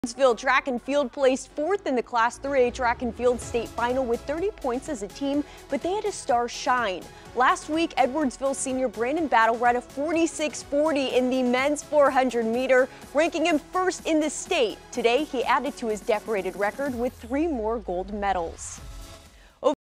Edwardsville track takes fourth in state, Battle adds to decorated record
Battle added to his standing as one of the greatest track athletes in Illinois history.